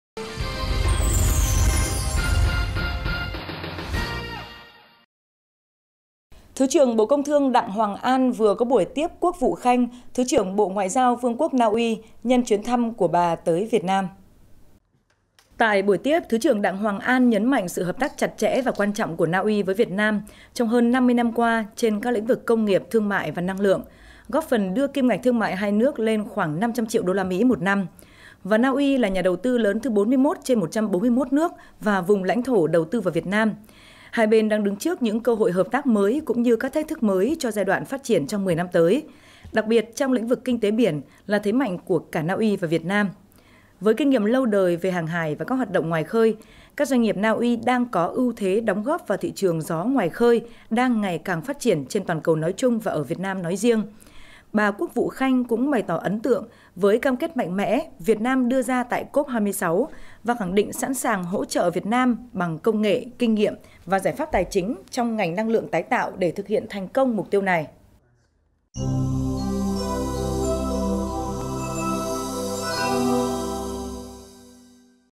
Thứ trưởng Đặng Hoàng An tiếp Thứ trưởng Bộ Ngoại giao Vương quốc Na Uy
Thứ trưởng Bộ Công Thương Đặng Hoàng An vừa có buổi tiếp Quốc vụ khanh, Thứ trưởng Bộ Ngoại giao Vương quốc Na Uy nhân chuyến thăm của Bà tới Việt Nam…